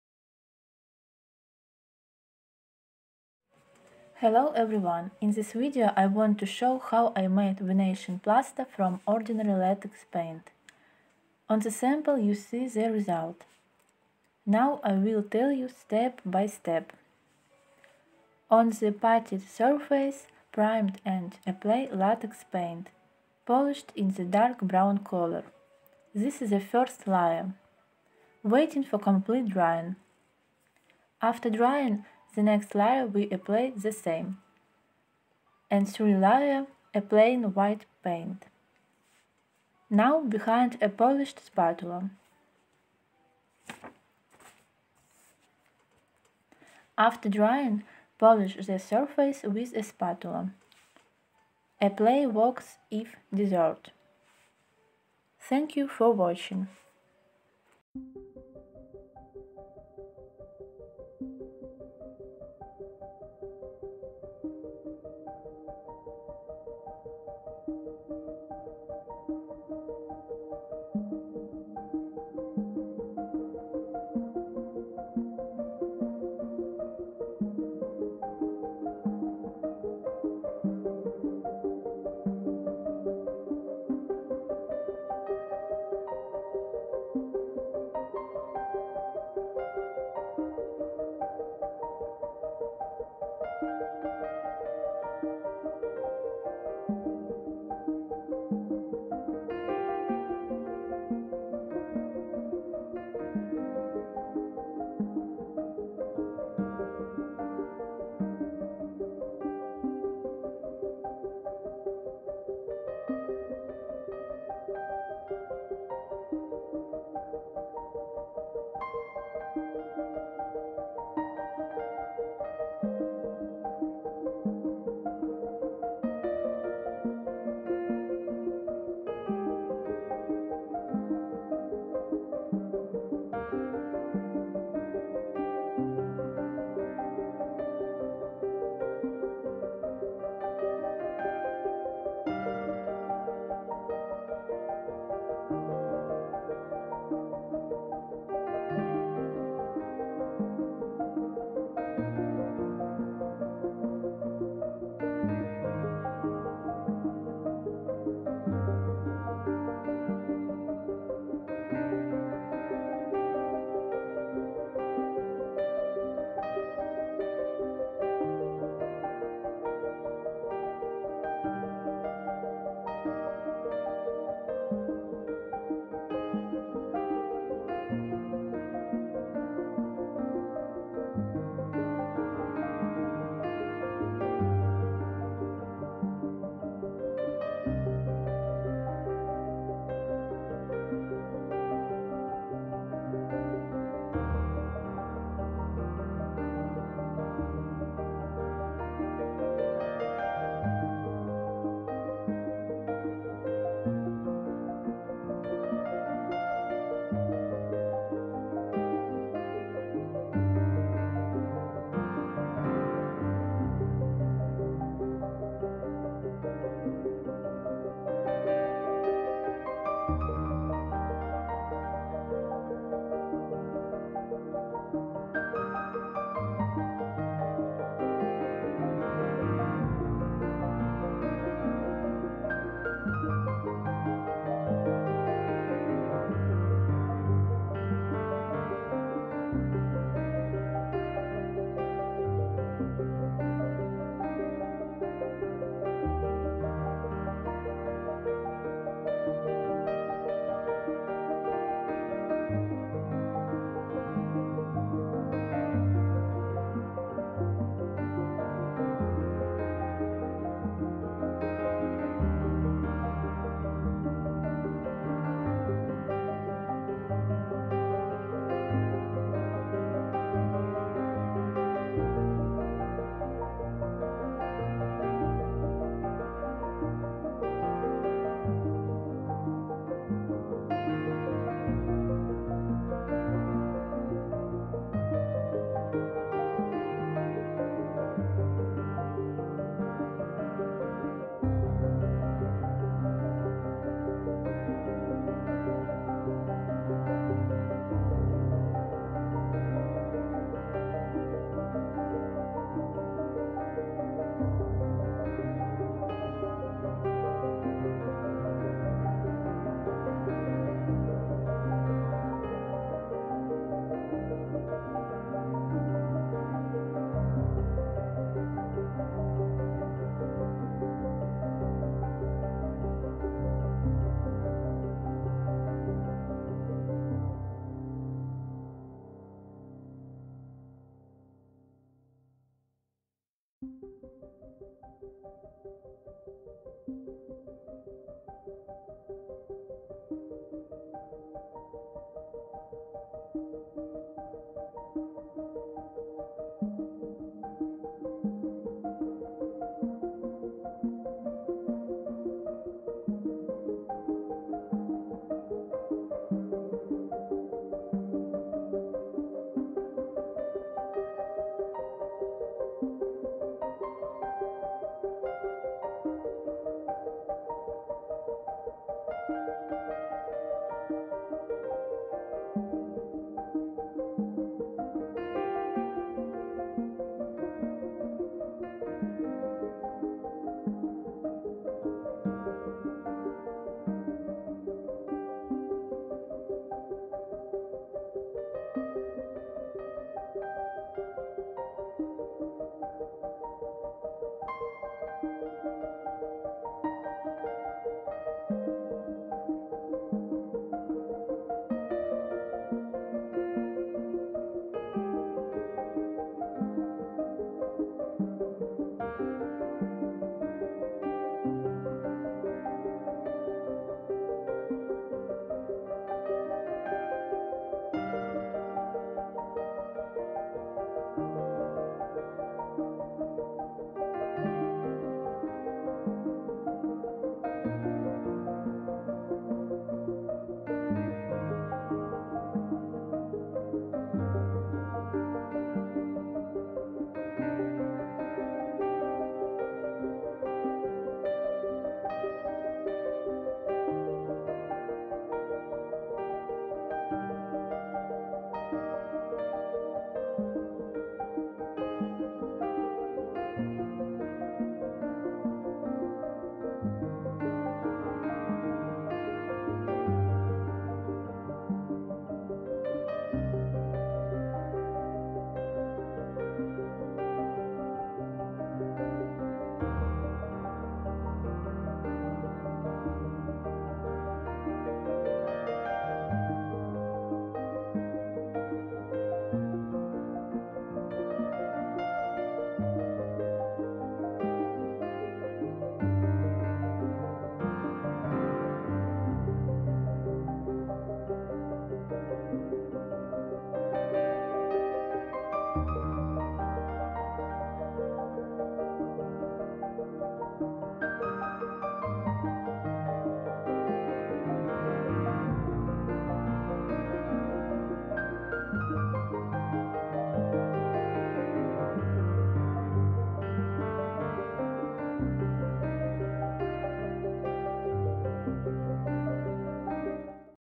Simple Decorative Plaster unusual Paints .How to do it yourself.
Decorative plaster from ordinary paint. Decorative Plaster is made from facade paint. We tint the paint in the color we want. On the puttied surface, apply the first layer of paint for the substrate. Then we take a thicker paint and make a drawing with a trowel. We heat it with a hair dryer for cracks, but this is optional ... After drying, we clean the tops with an emery boom. number 400-800 ... At the end, we polish everything with a spatula without pressing hard. Achieving darkening of the upper layer